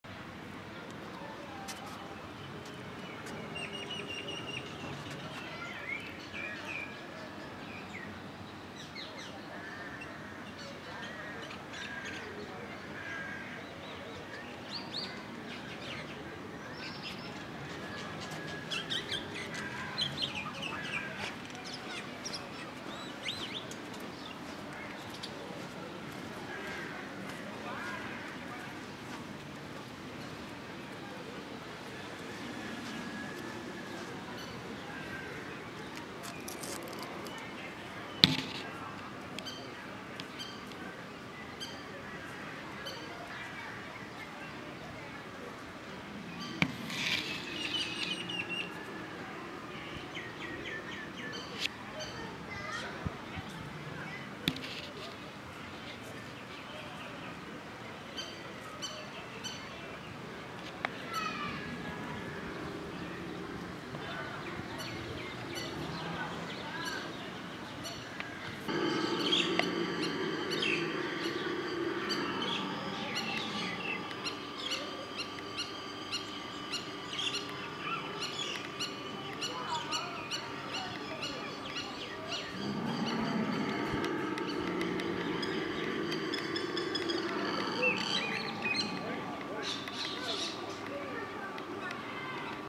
ישראל│Israel│קודו ניאלה│Lowland Nyala│Tragelaphus angasii│58
מְדִינַת יִשְׂרָאֵל
Država Izrael

חי פארק - קריית מוצקין
Zoološki vrt Hai Park - Kiryat Motzkin

Video fragmente sam snimio u aprilu, 2023. godine, kao nemontiran materijal.